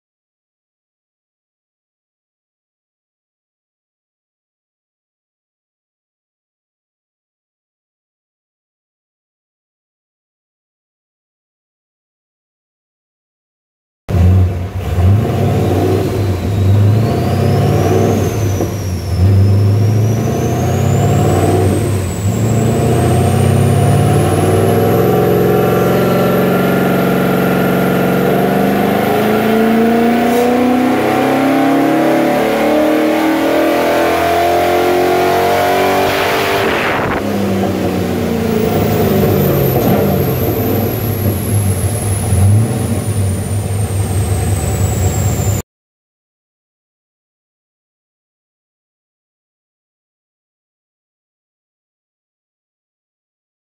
VN Group A Tribute Haltech upgrade
We fitted a Haltech GM platinum plug in to this very neat tribute VN 5.7L Holden commodre and tuned it on the dyno picking up power through the whole RPM range.